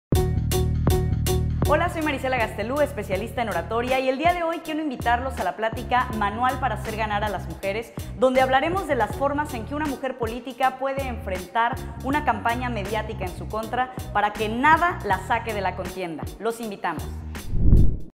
Maricela Gastelú - Manual para hacer ganar a las mujeres
Herramientas pensadas exclusivamente para mujeres políticas y que estén entrenadas para enfrentar una campaña mediática en su contra y que nada la saque de la contienda.